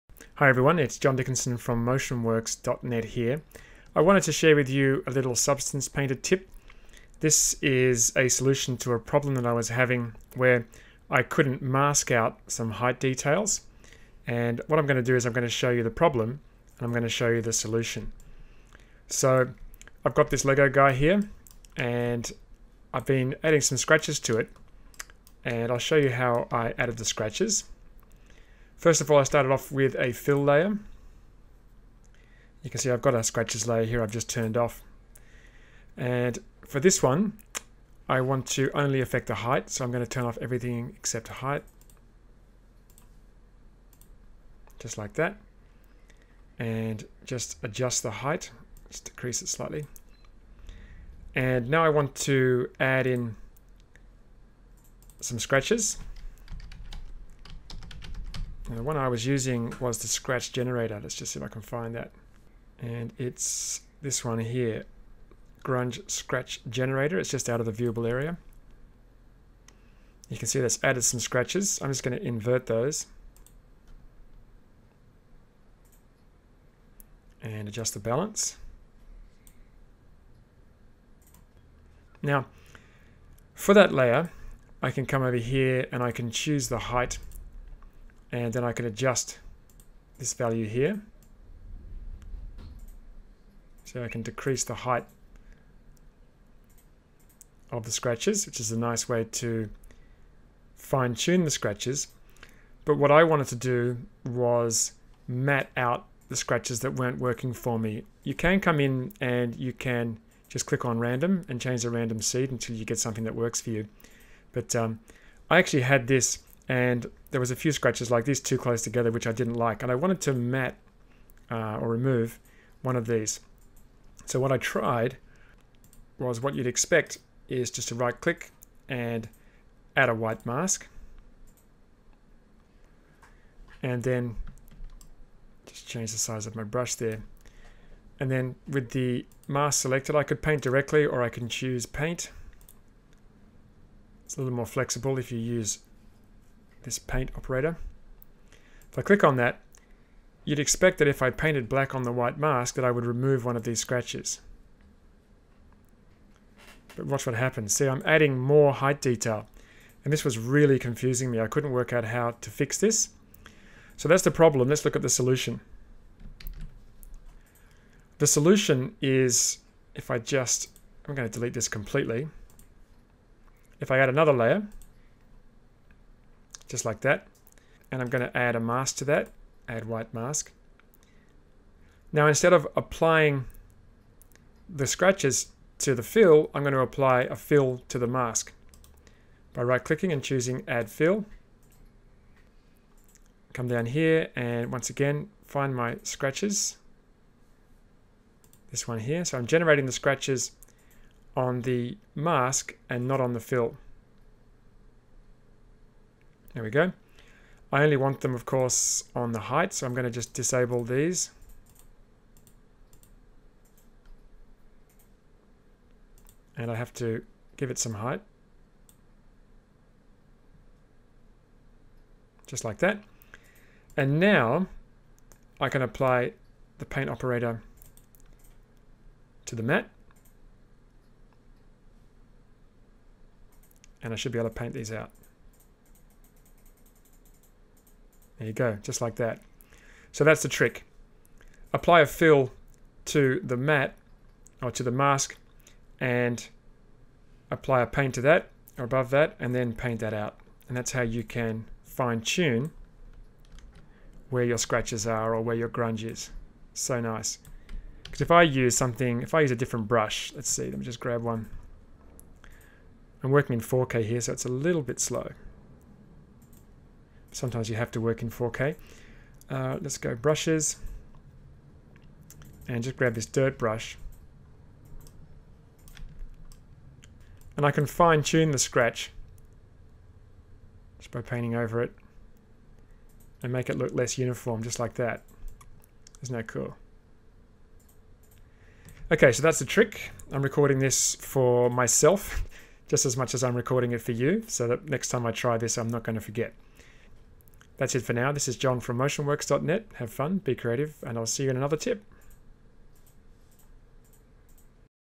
Masking Height Details in Substance Painter
A short tip on how to successfully paint out height details in Substance Painter.